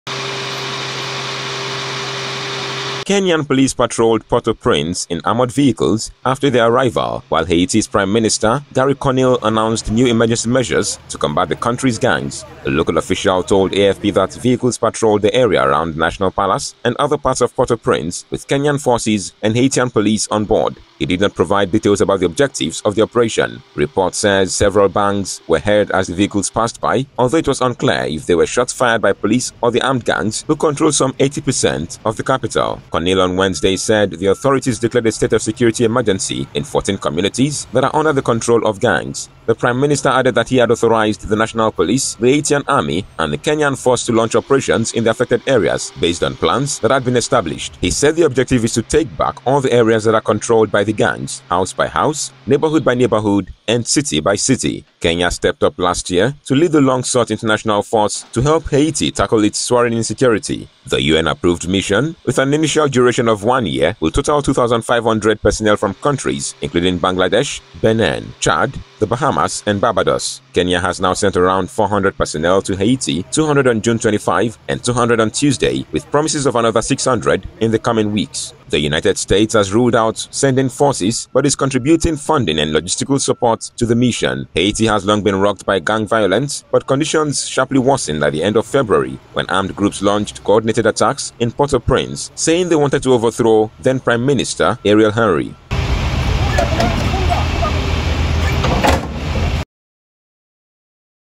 Kenya Police Patrol Haiti Capital After More Forces Arrive
Kenyan police patrolled Port-au-Prince in armored vehicles after their arrival, while Haiti's Prime Minister Garry Conille announced new emergency measures to combat the country's gangs.